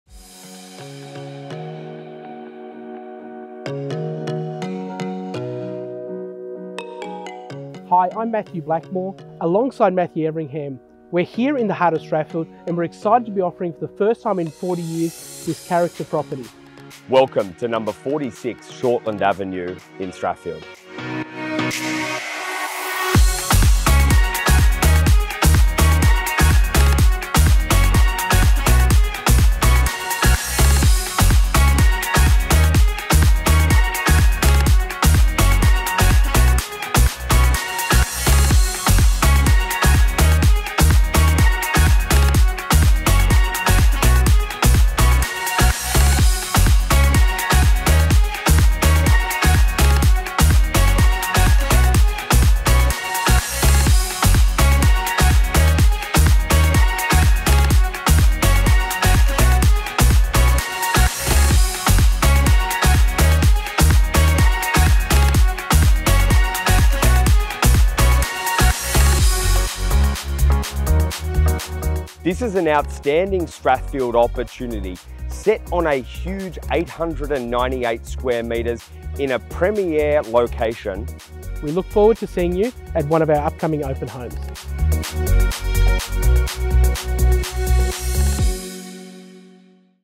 [TeamRichardMatthews] 46 Shortland Avenue, Strathfield with Matthew Blackmore & Matthew Everingham
TeamRichardMatthews are proud to present to market 46 Shortland Avenue, Strathfield with Matthew Blackmore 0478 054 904 & Matthew Everingham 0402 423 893

Richard Matthews Real Estate | Strathfield | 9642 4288
Richard Matthews Real Estate | Summer Hill | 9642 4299
Richard Matthews Real Estate | Georges Hall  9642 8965